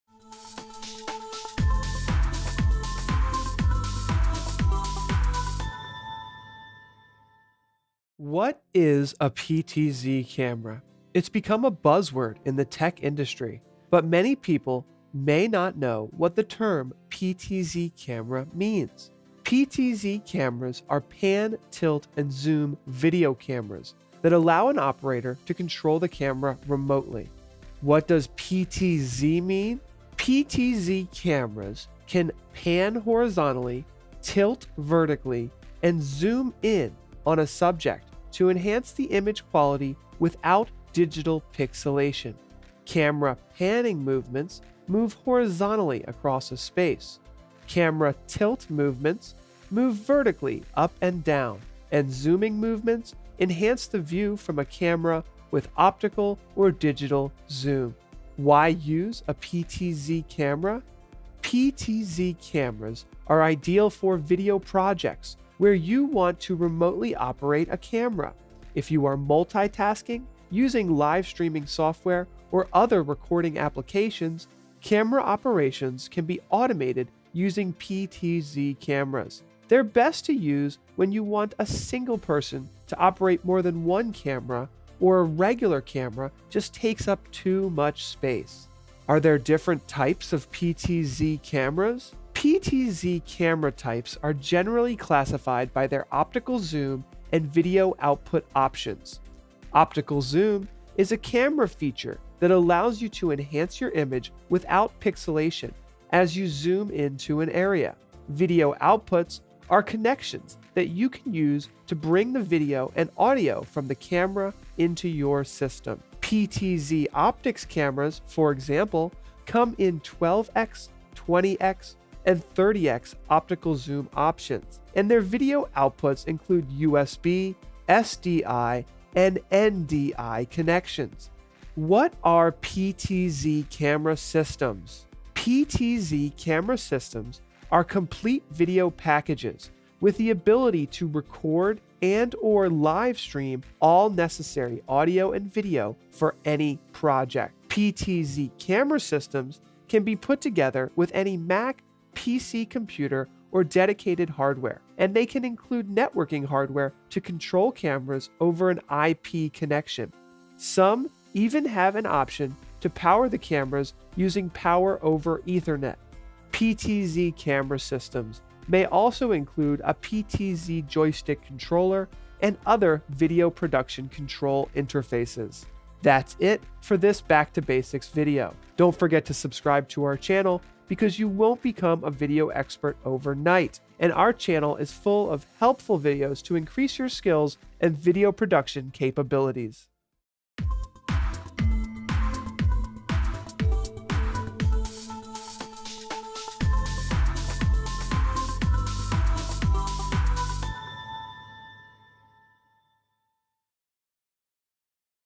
What is a PTZ camera?
VIDEO INDEX:
0:08 - What is a PTZ camera?
0:27 - What does PTZ mean?
0:58 - Why should I use a PTZ camera?
1:28 - Are there different types of PTZ cameras? (SDI, NDI, etc.)
2:11 - What are PTZ camera systems?